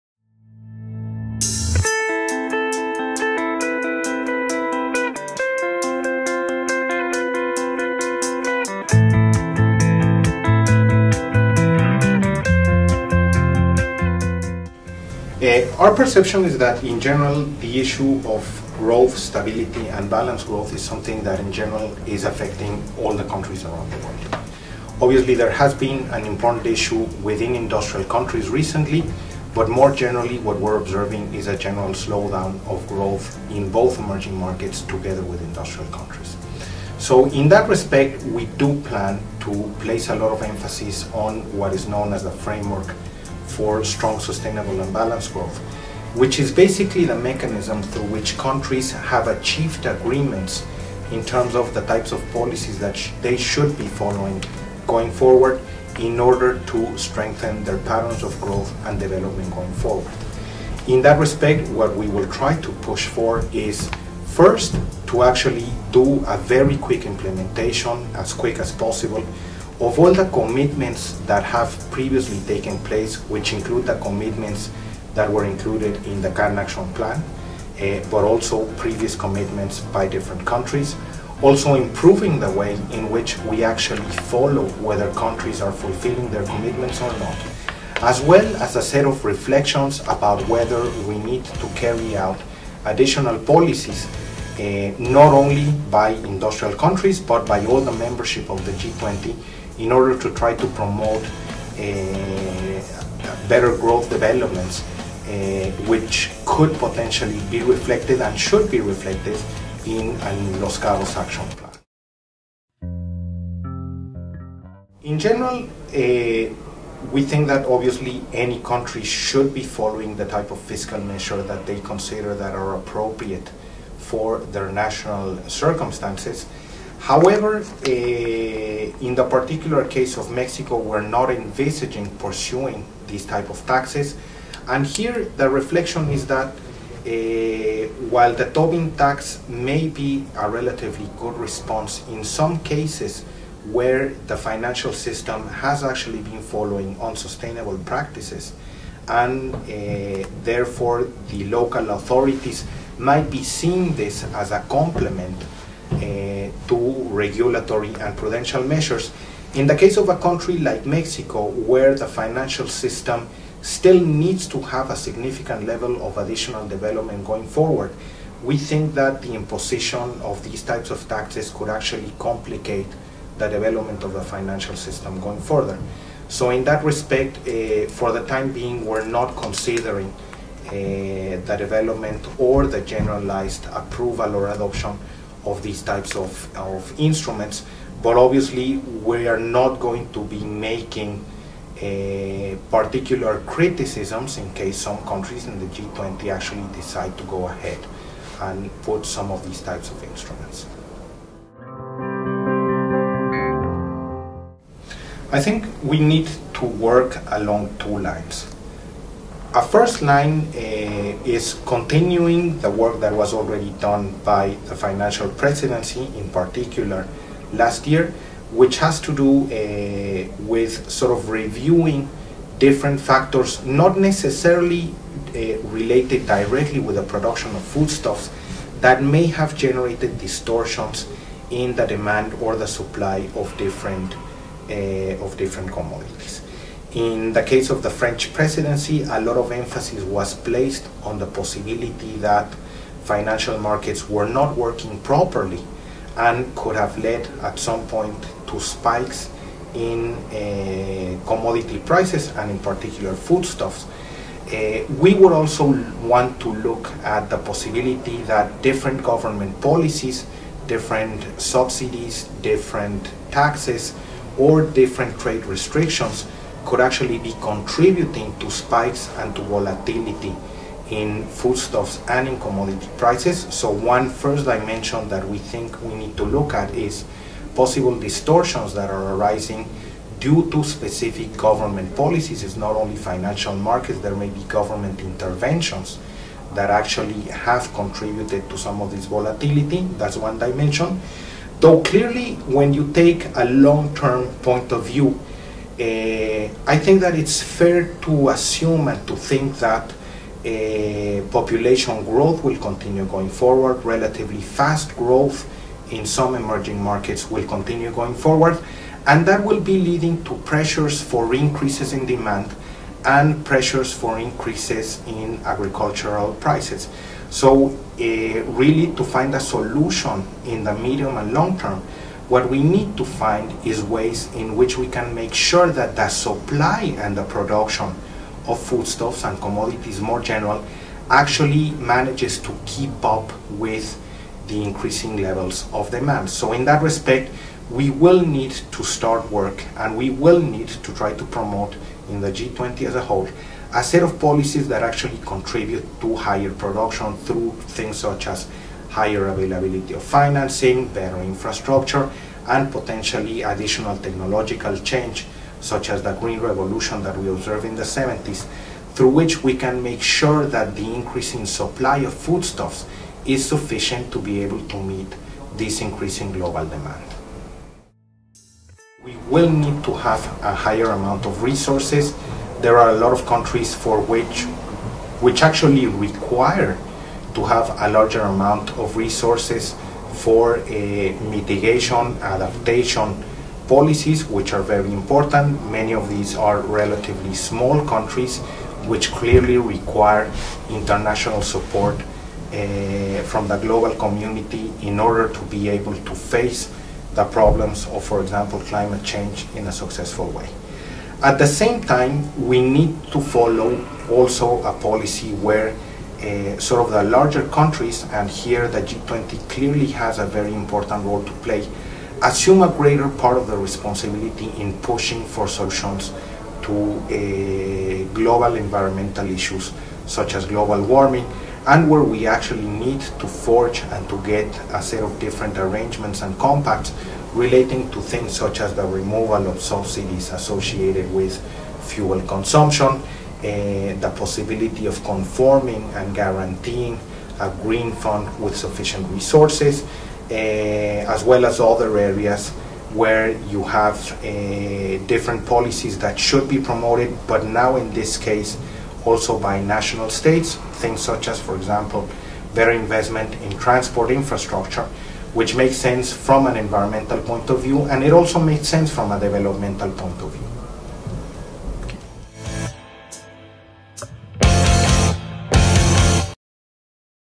Club de Madrid-G20 Initiative: Interview to Dr. Messmacher
Following our initiative on 'The Political Dimensions of the World Economic Crisis 'whereby the effects of the crisis on good governance were explored through different regional roundtables, Club de Madrid members decided to contribute towards the restructuring of the international governance architecture, particularly, by supporting the G20 in a long-term and sustainable approach, focused in bringing about a more efficient, effective and representative system capable of responding to the realities and challenges of the 21st century.

This year, we will be working with the Mexican Presidency of the G20. On February 7th, 2012, the Club de Madrid and the Mexican Embassy in Spain, co-organized a roundtable with Dr. Miguel Messmacher Linartas, Titular de la Unidad de Planeación Económica de la Hacienda Pública from Mexico, to discuss the priorities of the Mexican Presidency of the G20.

Don't miss this interview!